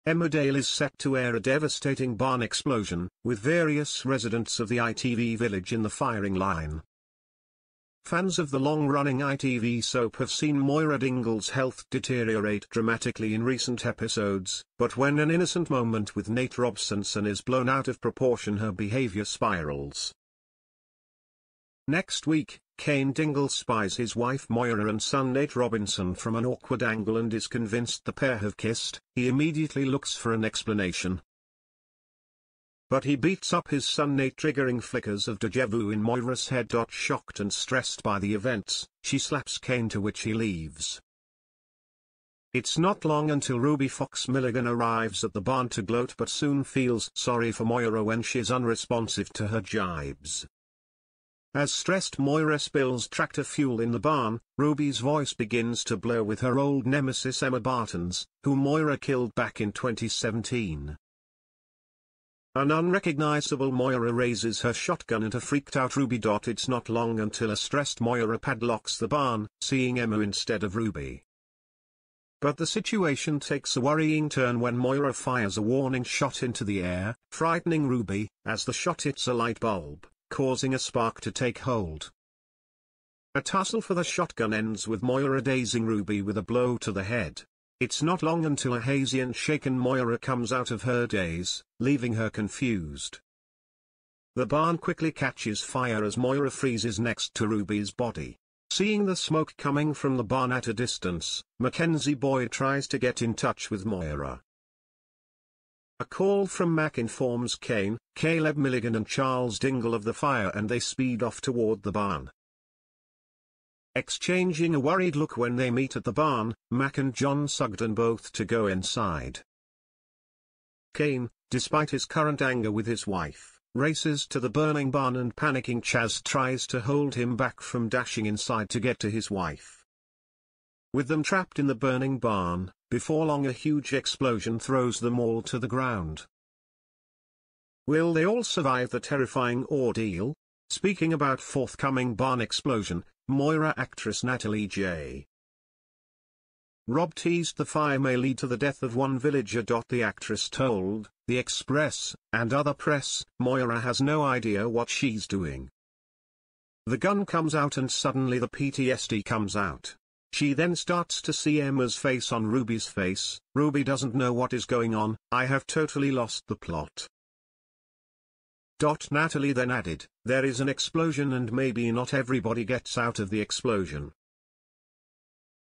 Emmerdale barn bIaze reveaIed - Emma Barton return, death tragedy and gun drama
But he beats up his son Nate triggering fIickers of de ja vu in Moira's head.